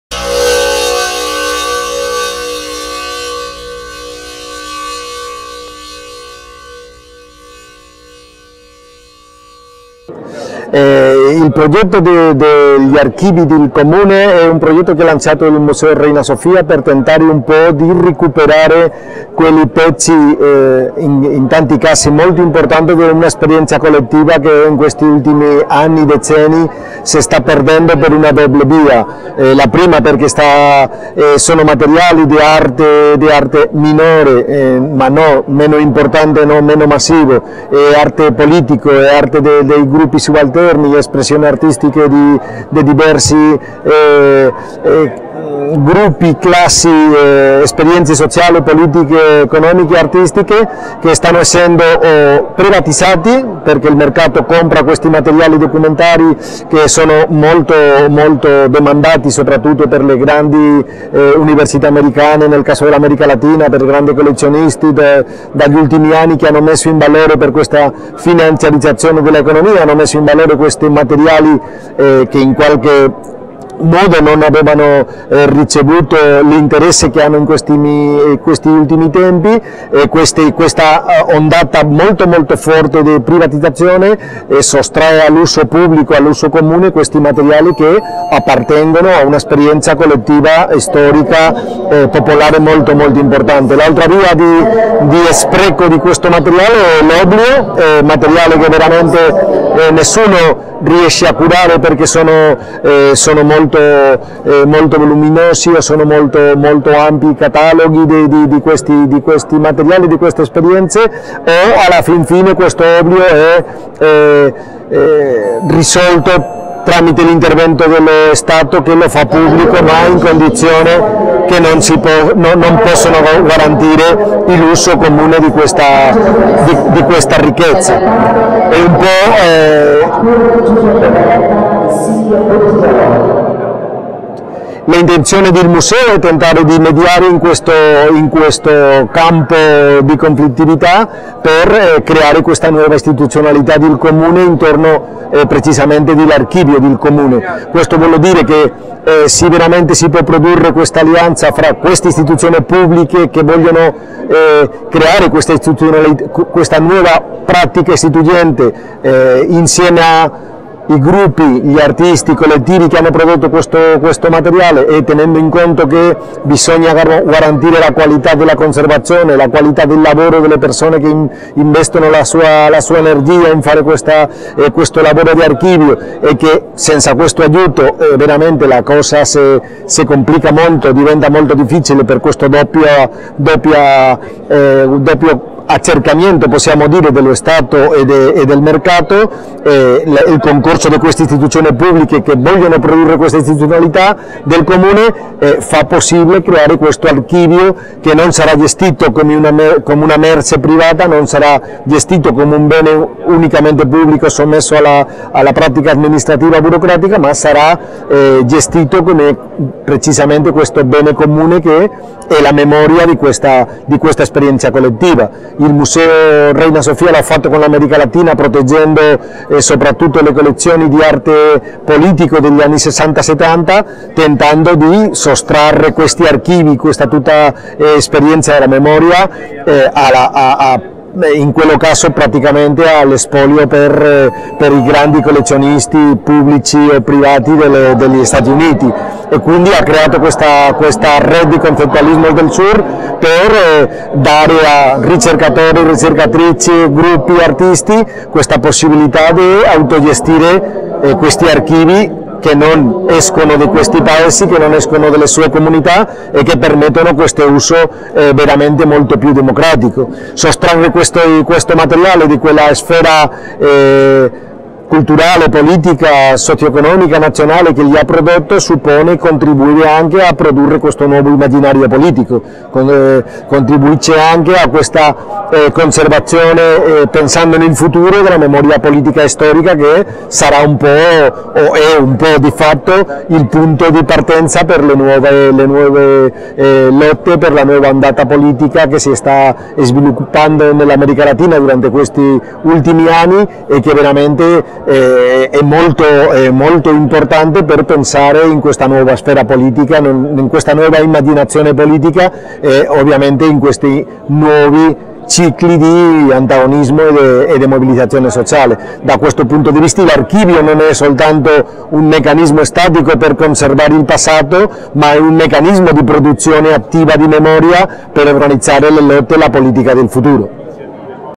Documentario e Archivio nell’arte contemporanea
Bellissima Fiera 2016
Milano, 19 marzo 2016 | Frigoriferi Milanesi

Tavola rotonda con la partecipazione di Marco Scotini (Nuova Accademia di Belle Arti, Milano), Elena Volpato (GAM, Torino) Carlos Prieto del Campo (Museo Nacional Centro de Arte Reina Sofia, Madrid), Paolo Caffoni (Archive books, Berlino).

Documentario e Archivio nell’arte contemporanea. Marco Scotini, Elena Volpato, Carlos Prieto del Campo e Paolo Caffoni discutono delle pratiche dell’archivio e del documentario nell’arte contemporanea, in occasione della pubblicazione in inglese per  la casa editrice Archive Books del volume Politics of Memory (2016), pubblicato in Italia da Derive Approdi (Politiche della memoria, 2013). È previsto uno screening dei film di alcuni degli artisti e documentaristi presentati nel volume: Deimantas Narkevičius, Gintaras Makarevičius, Angela Melitopoulos e Clemens von Wedemeyer.